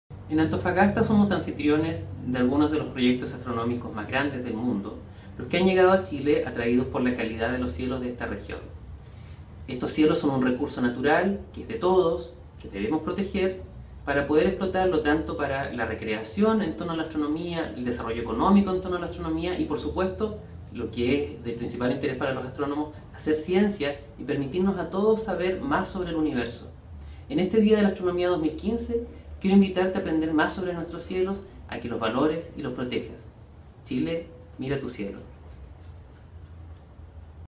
Eduardo Unda-Sanzana. Los Cielos de Chile
El profesor Unda-Sanzana es astrónomo de la Universidad de Antofagasta